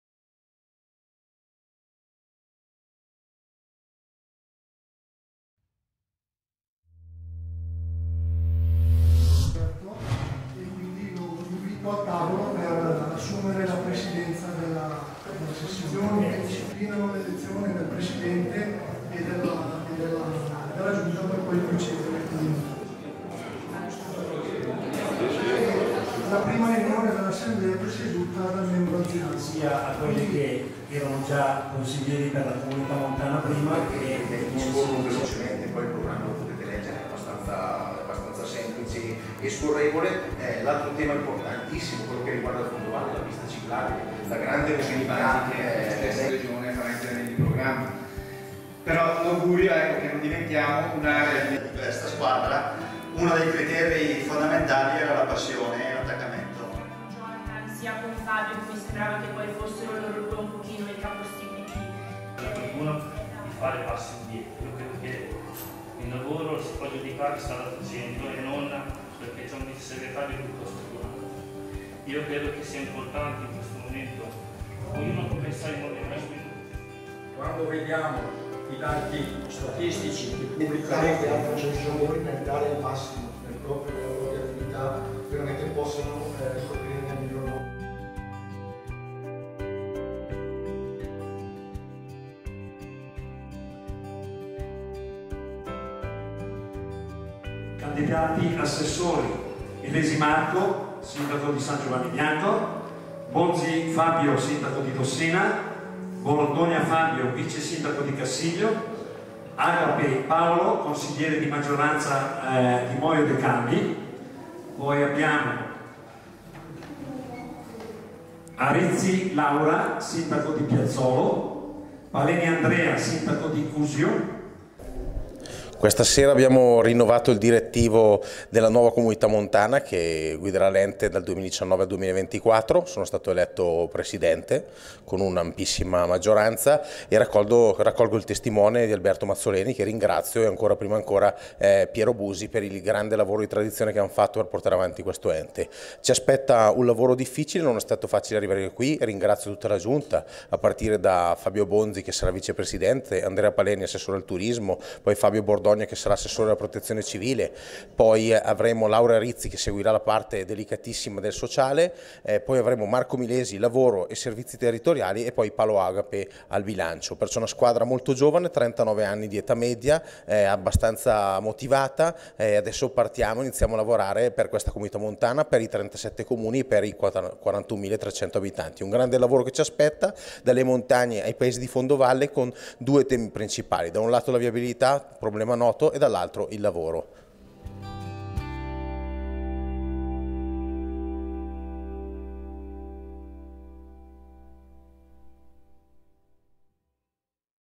Jonathan Lobati, è il nuovo presidente della Comunità Montana Valle Brembana
Piazza Brembana, 25 Luglio 2019 - Comunità montana Valle Brembana, Jonathan Lobati è il nuovo presidente: 28 favorevoli, tra gli astenuti Zogno e Piazza Brembana, contrario San Pellegrino. Cinque i Comuni assenti alla seduta: Branzi, San Giovanni Bianco, Sedrina, Valleve e Valnegra.